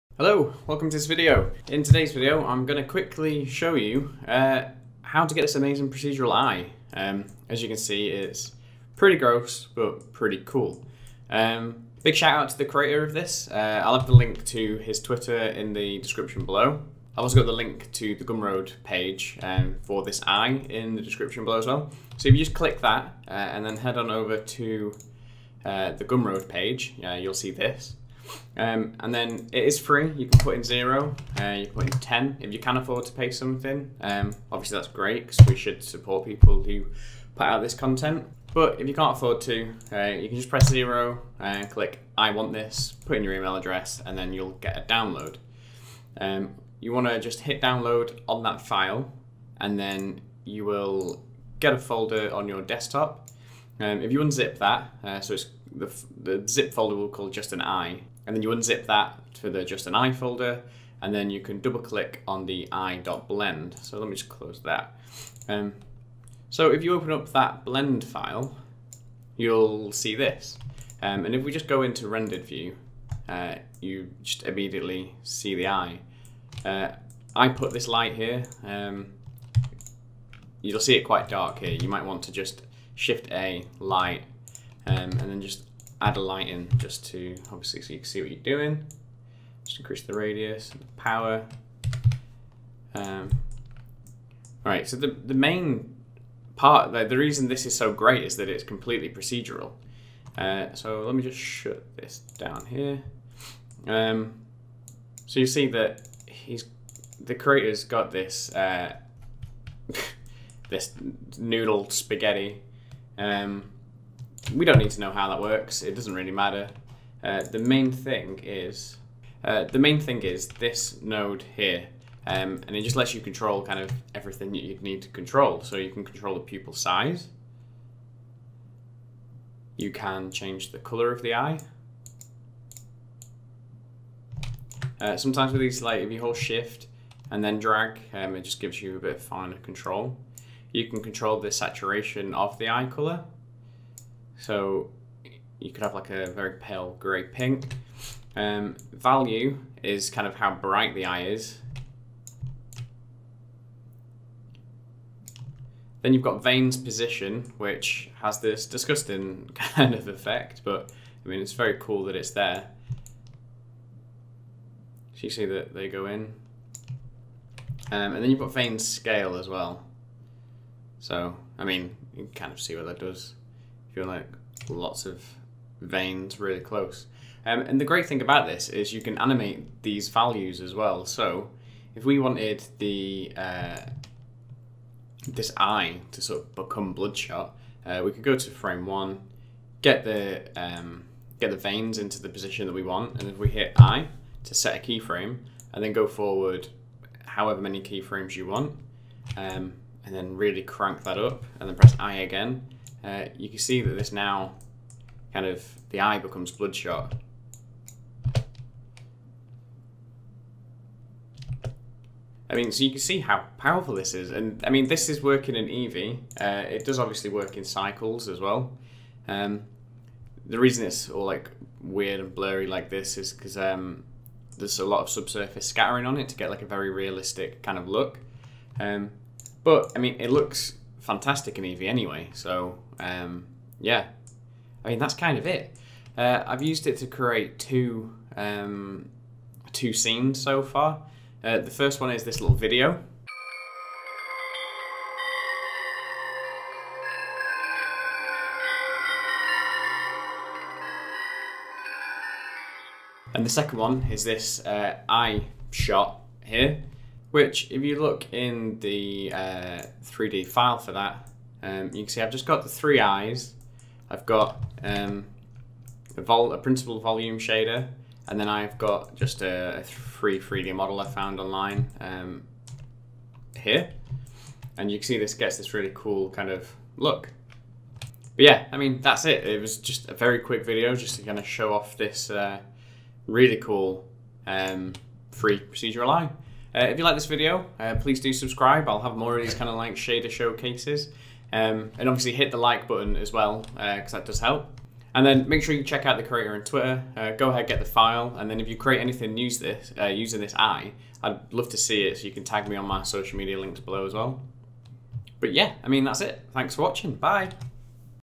Blender Eye Tutorial: This FREE PROCEDURAL EYE SHADER works with EEVEE - Blender Eye Shader Showcase
🚨 Wishlist Revolocity on Steam! 🚨 This Blender eye tutorial will show you how to get a free procedural eye shader which you can use in your projects. This procedural eye shader has a range of settings that mean you can get a range of beautiful or creepy eyes in blender - all for free!